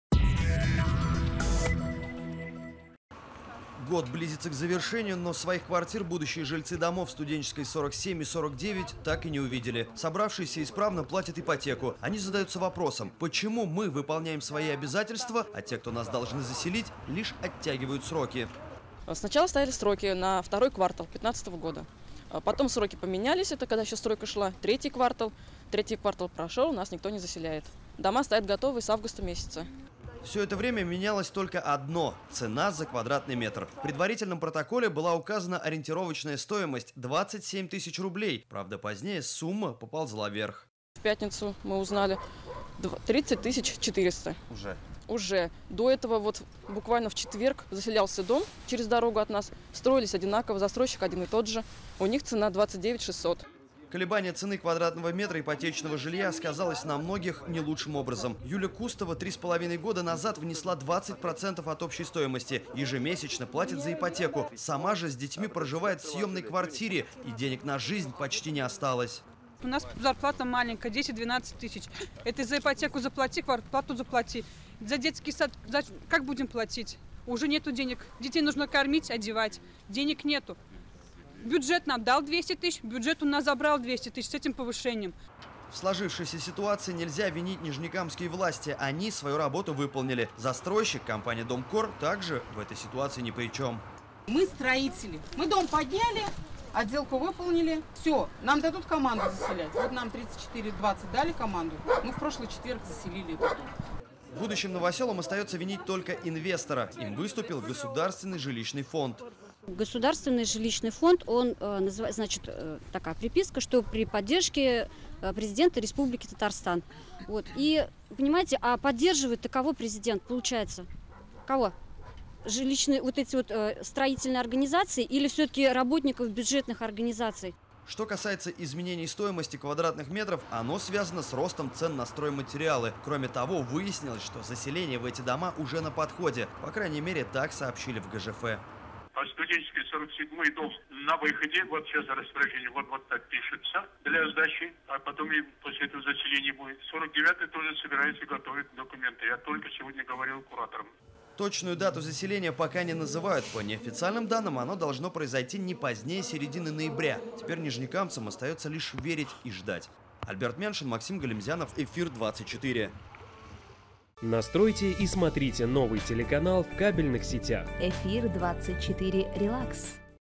В Нижнекамске бюджетники не могут заселиться в квартиры, взятые в ипотеку
Получить ключи новоселы домов №47 и №49 по улице Студенческая должны были еще прошлой весной.